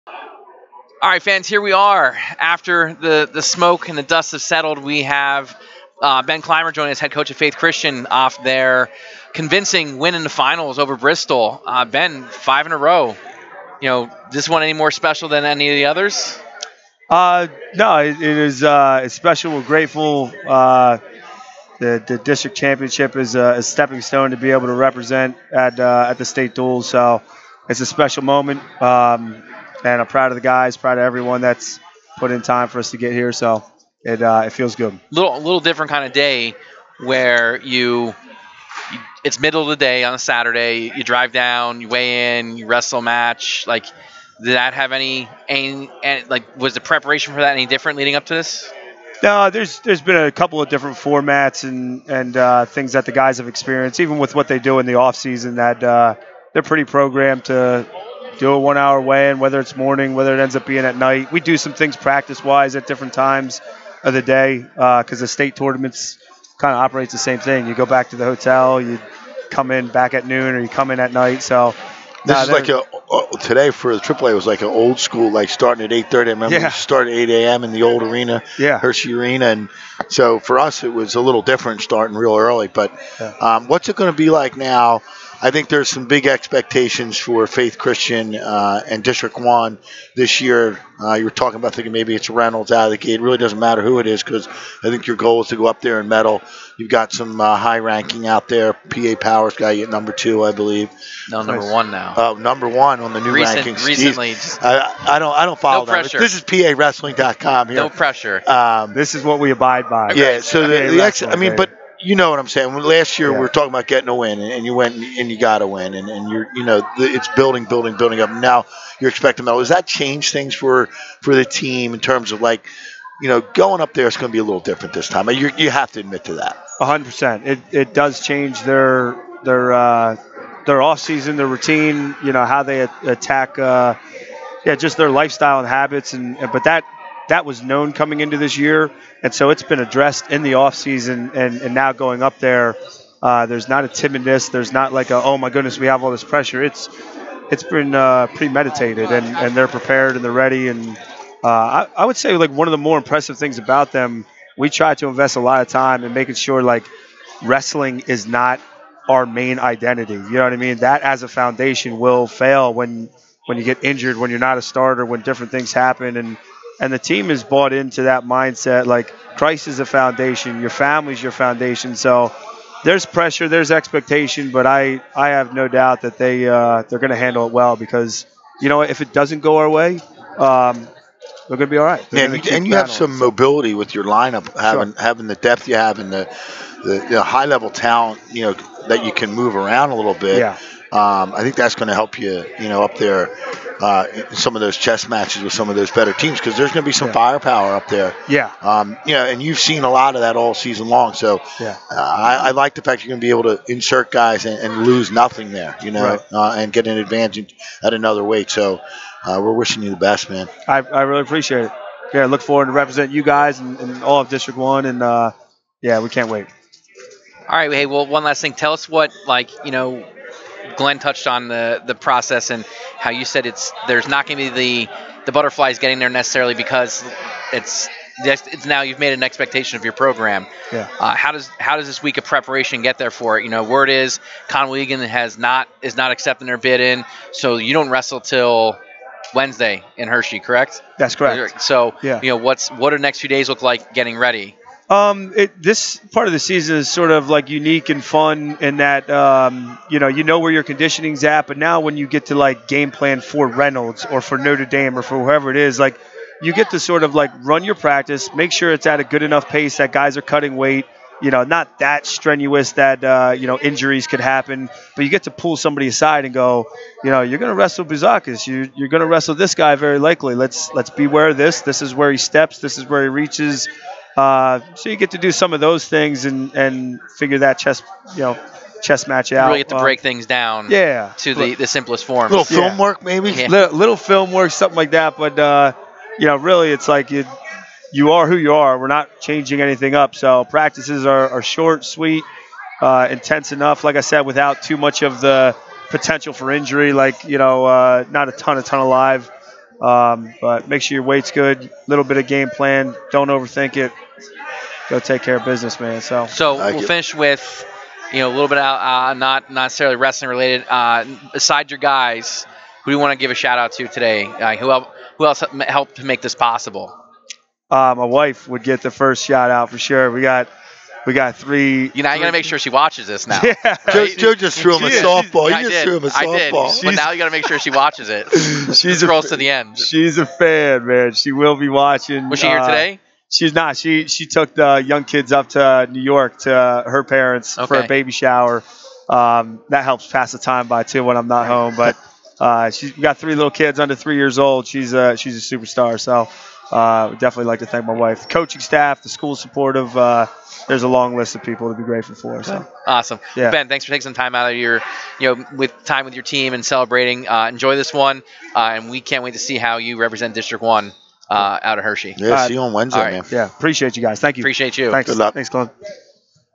2023-02-04 Interview with Faith Christian head coach Ben Clymer
Joe Youngblood and Glenn Kaiser sit down with Faith Christian Academy head coach Ben Clymer after his team wins their 5th straight District 1 AA Duals championship on Saturday, February 4, 2023.